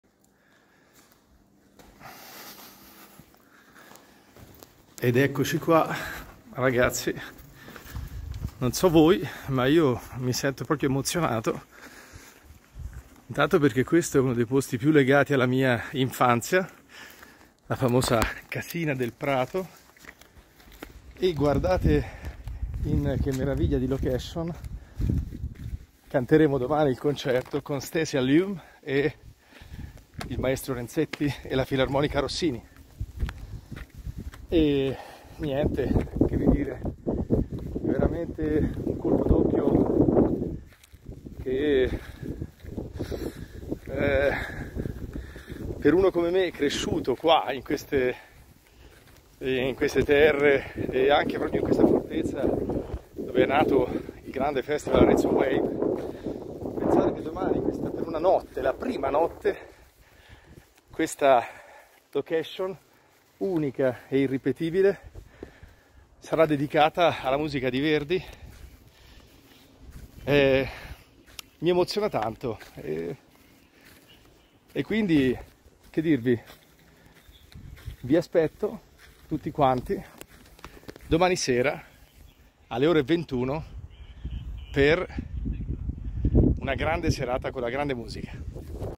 Concert venue tour!
Introducing a concert venue, Fortezza Medicea in Arezzo, on a day before the concert.

♬  Arezzo concert
🏫 Fortezza Medicea
🇮🇹 Arezzo, Italy

CONDUCTOR : Donato Renzetti
SOPRANO : Stacey Alleaume
BARITONO : Mario Cassi
ORCHESTRA : Filarmonica Gioachino Rossini